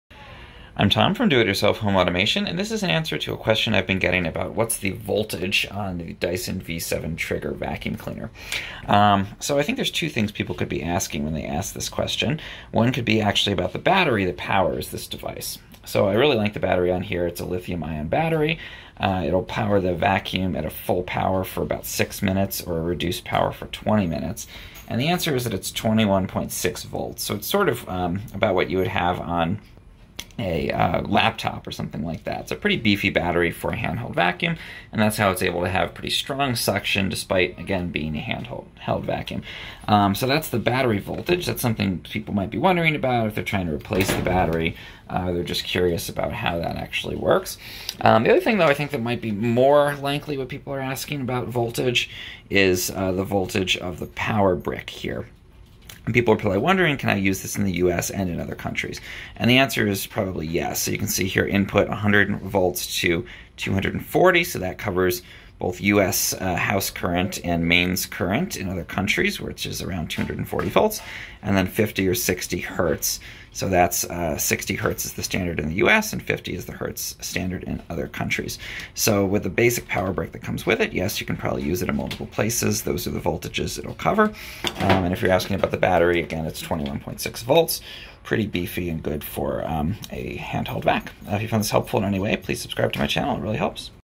What Voltage Is Dyson V7?
Looking at the voltage of the Dyson V7 Trigger vacuum cleaner, both for the battery and the charger, as well as if the charger works internationally, January 4, 2020.